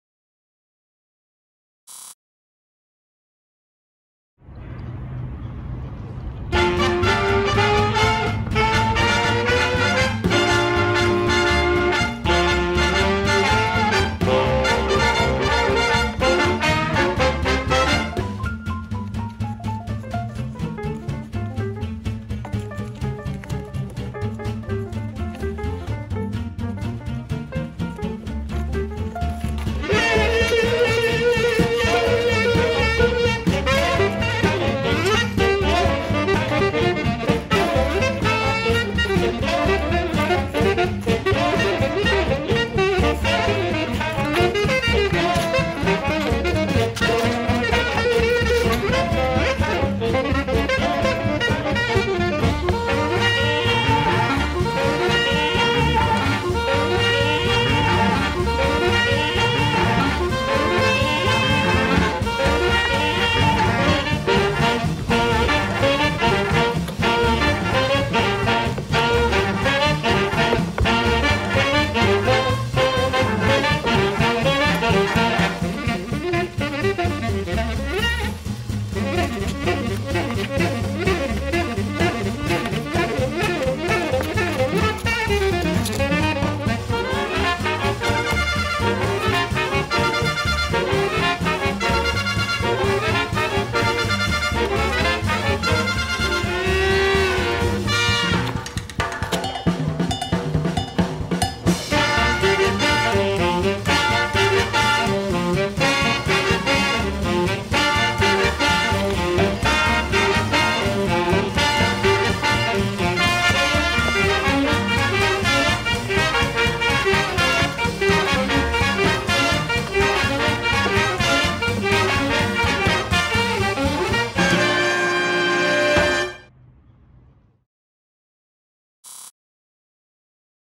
ILHC 2020 - Video Showcase - Kelly Arsenault & Mickey Fortanasce (United States)
As first seen at Virtual ILHC 2020. The International Lindy Hop Championships takes place, in person, each year in Washington, DC. In 2020, ILHC held a virtual online event. for more information.

Competitors:
1 - Michael Tan & Natasha Shevchenko (United States)
2 - Yeon-Ok Park & Seonghwan Jeong (South Korea)
3 - Antoine Simard & Coralie Bazinet (Canada)
4 - Aurore Alauze & Thomas Latter (Hong Kong)
5 - Kelly Arsenault & Mickey Fortanasce (United States) **2nd Place**
6 - Celeste Plaza & Mariano Ballesteros (Argentina)
7 - Song Hi & Jason (South Korea)
8 - SwingTopia (China) **1st Place**
9 - Youpi (France) **3rd Place**
10 - Pak Ipsae & Jung Gilsu (South Korea)
11 - Nakaji and Yusuke (Japan)
12 - Charlotte Zazou & Jethro Hardinge (Australia)